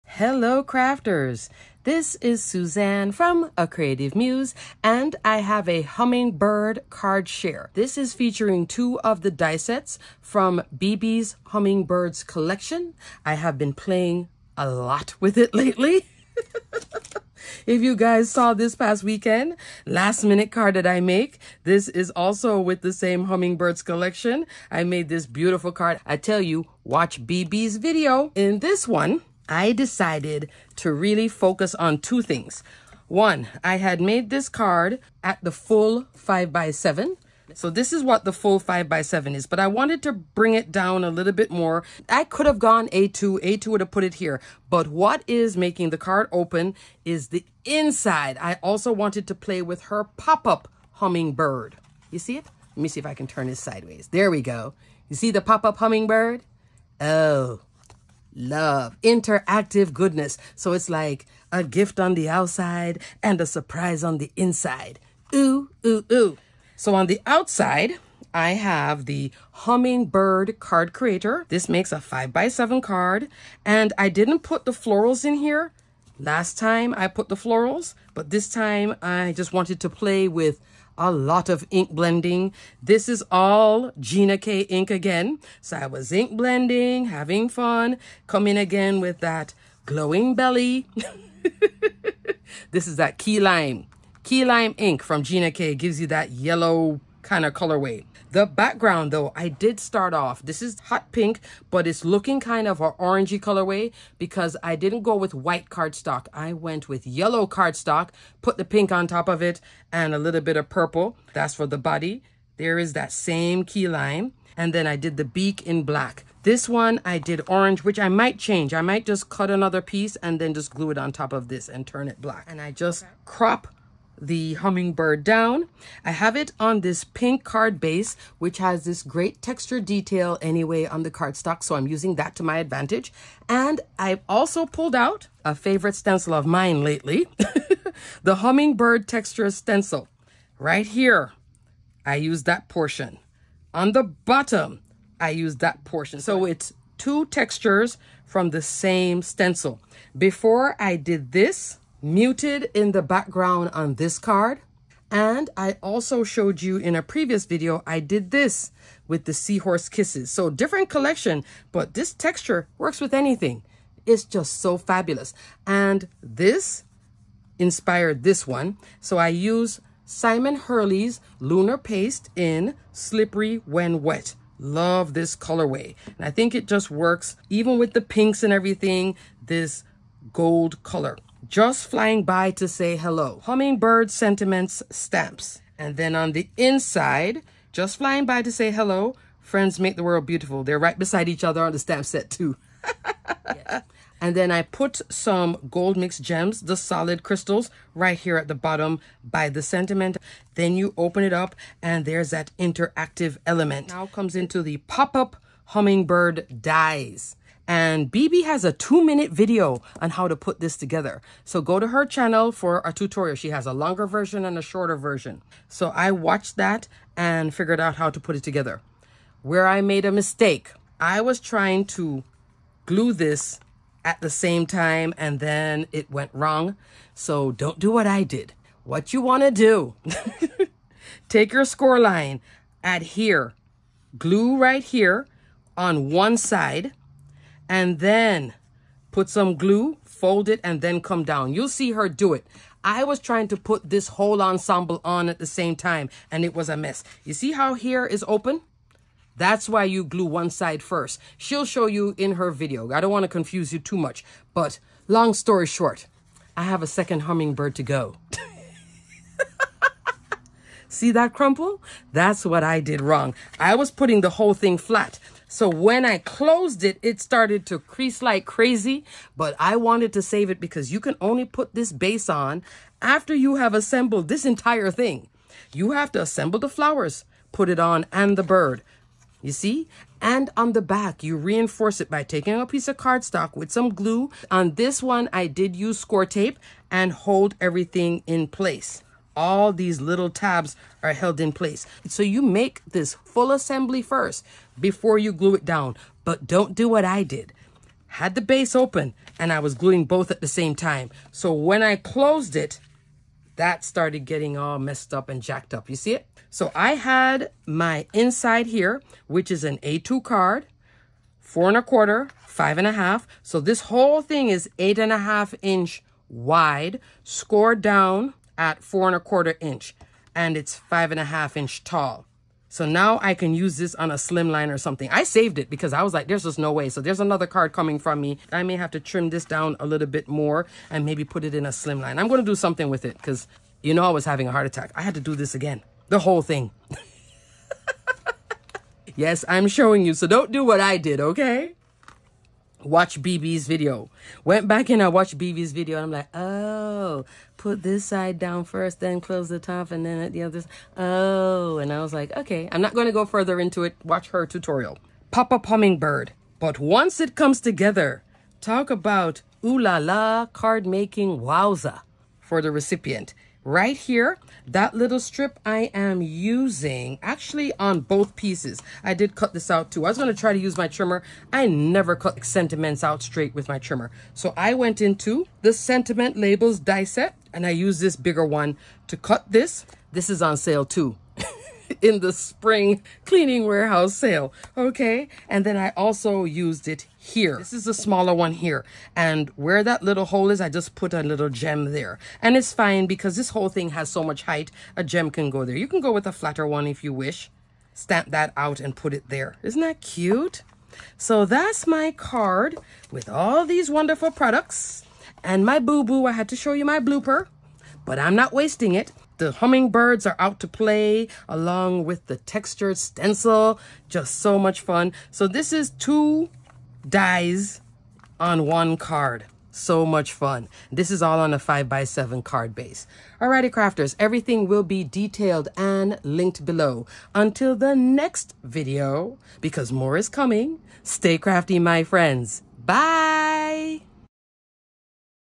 Pop-Up Card Share w/ Bibi's Hummingbird Collection
I made an interactive pop-up card with some great die sets from the Bibi's Hummingbirds Collection at Spellbinders! Let your cards take flight! Enjoy!
*** Product Details Listed Below ***

*** Free Glimmer Workshops | Watch & Play May 13th at 9AM PST | 12PM EST ***
You can buy kits if you want to follow along with goodies used in the workshop 😊👍

Age Restriction: This video is NOT intended for children under 18 years old.